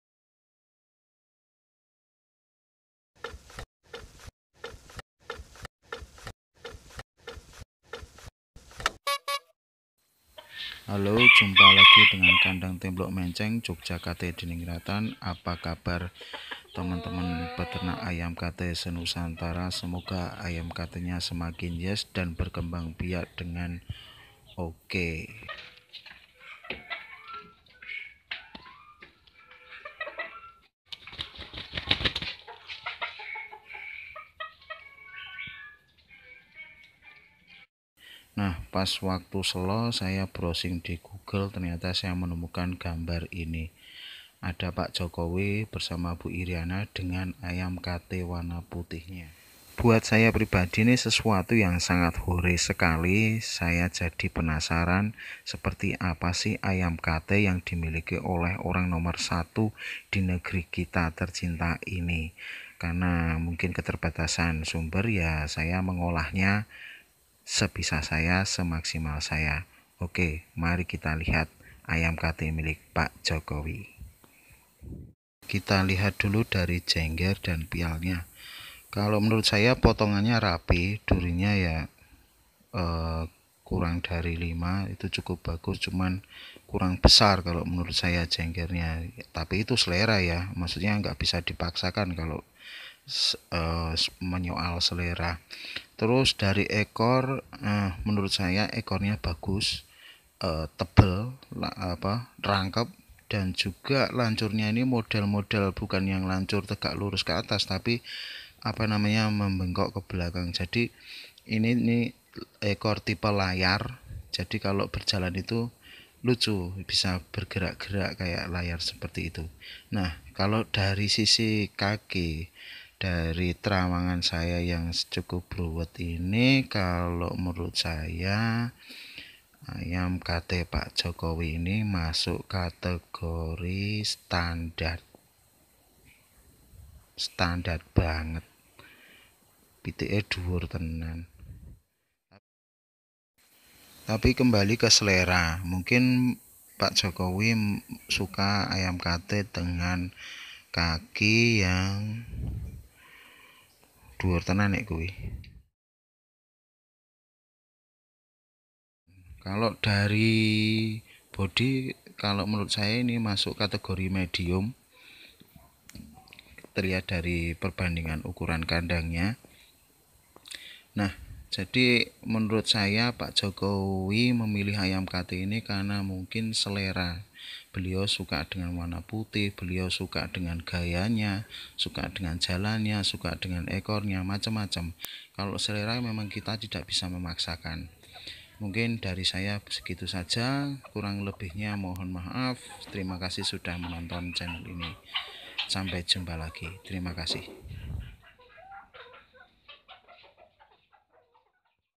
KATE PAK JOKOWI - Standar Nasional Indonesia
Berikut adalah video tentang rasa penasaran saya saja pada ayam kate milik Pak Jokowi. Sebagai arsip visual bahwa Orang Nomor Satu di Negeri Tercinta kita ini pelihara ayam kate.

Kalo kamu?